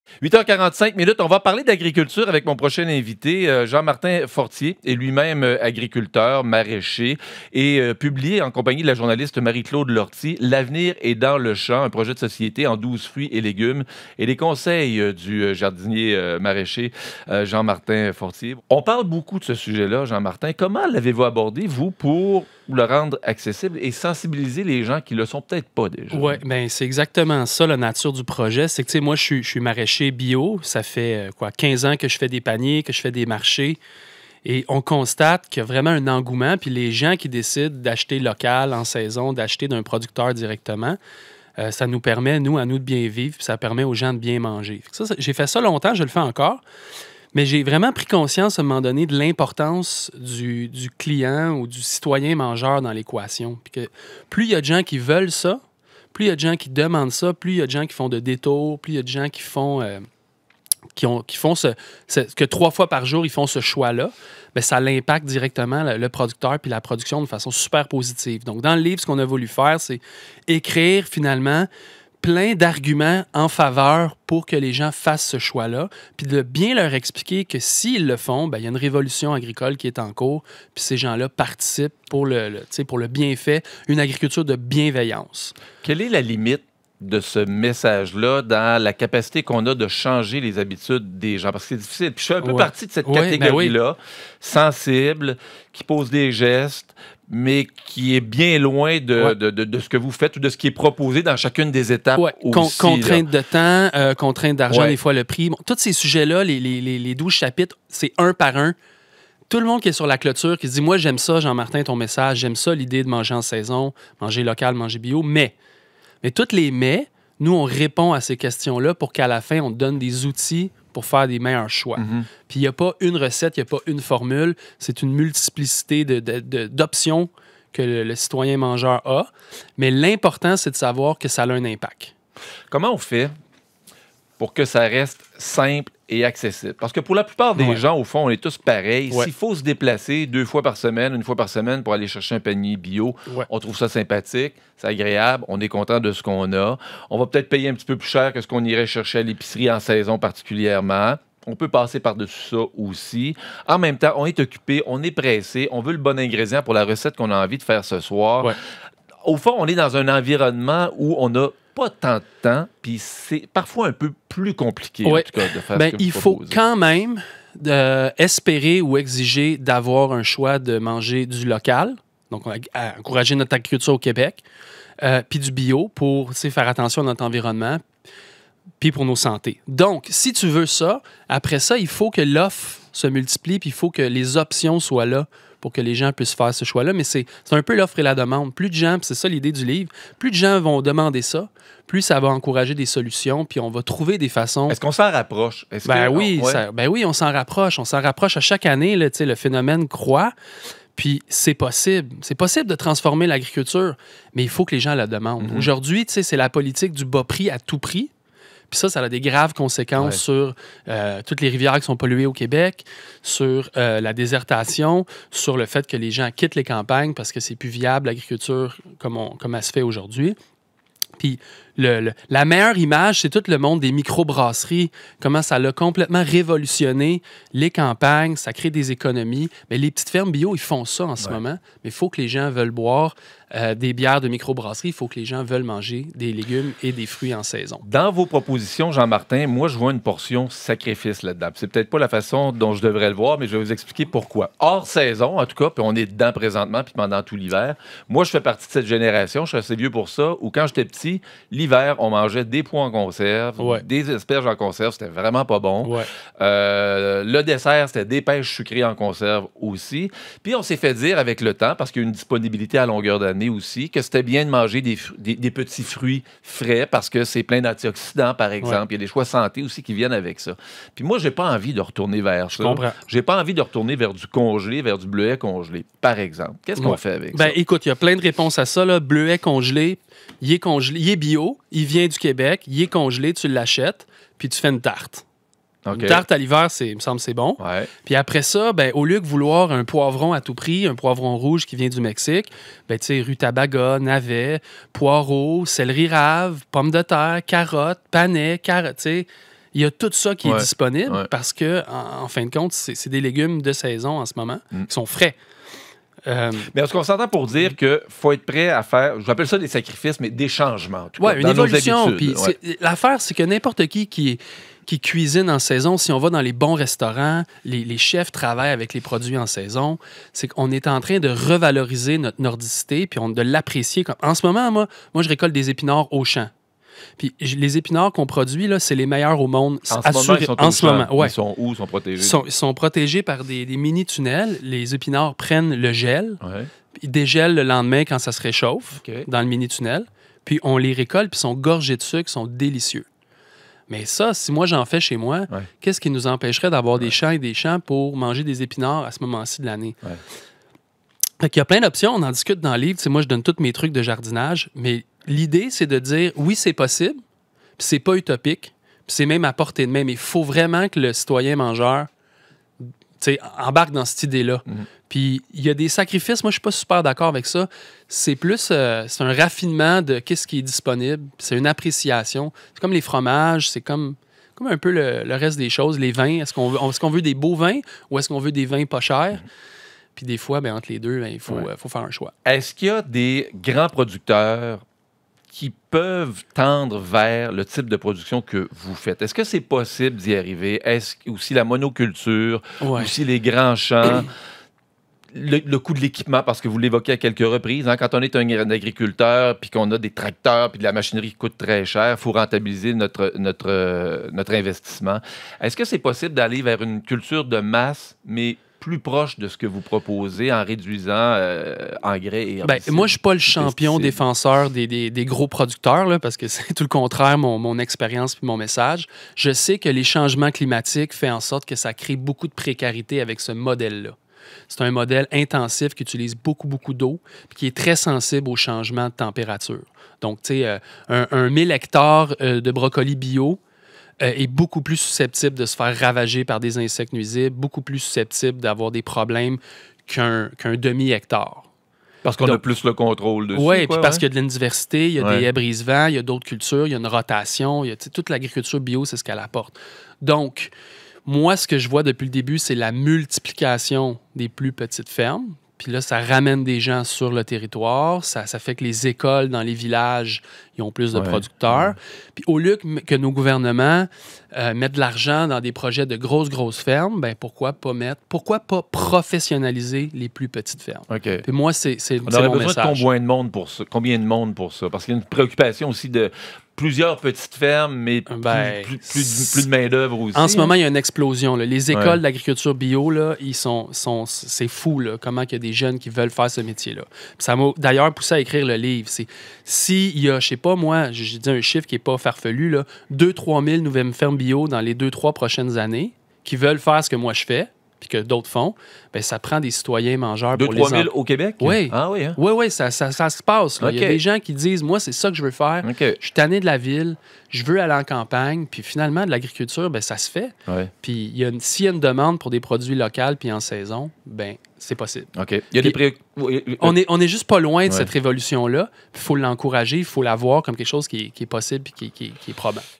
Une révolution alimentaire
Le jardiner-maraîcher Jean-Martin Fortier signe avec Marie-Claude Lortie « L'avenir est dans le champ», un projet de société expliqué en 12 légumes.